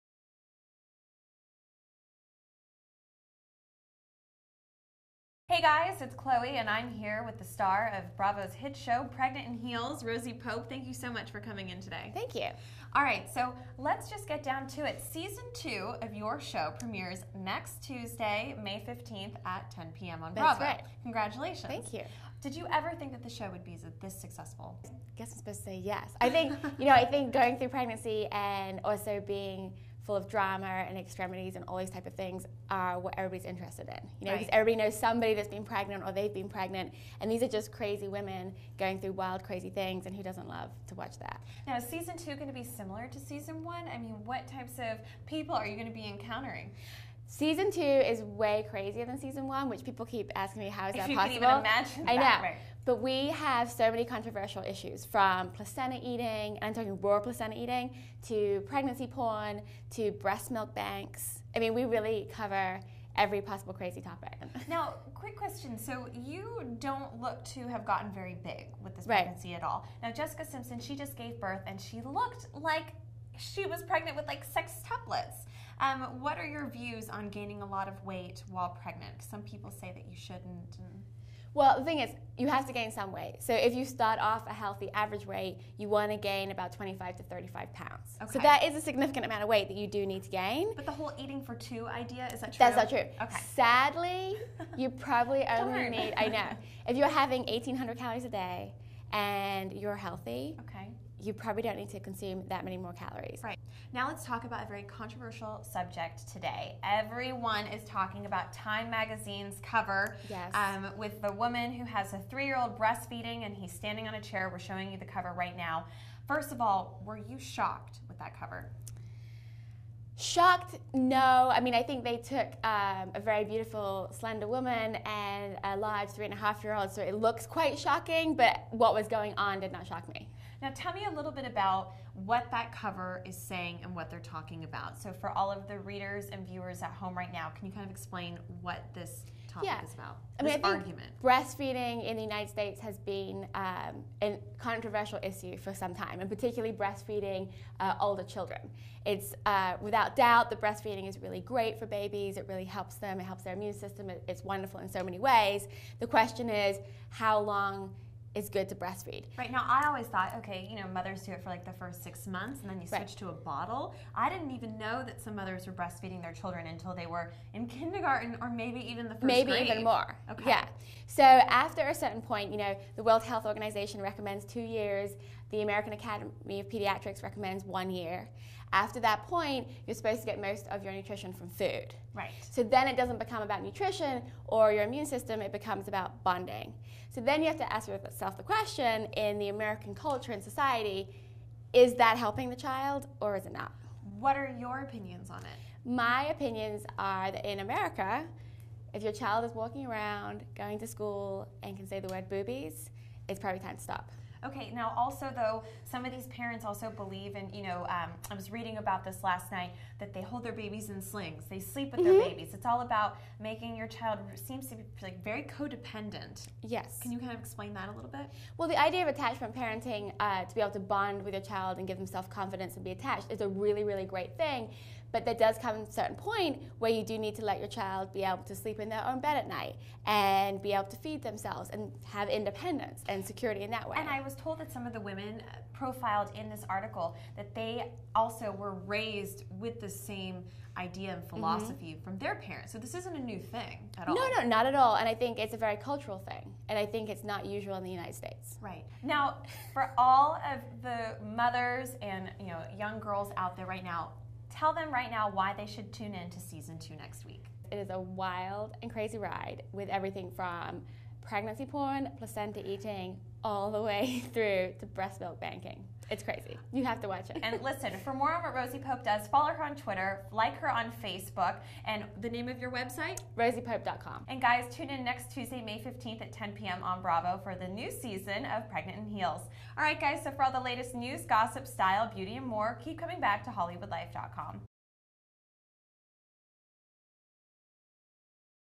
Rosie Pope 'Pregnant In Heels': Talks Season Two & TIME Cover
Rosie Pope came by the HollywoodLife.com offices to talk about the upcoming season two premiere of 'Pregnant in Heels' on May 15 on BRAVO and the controversial TIME magazine cover.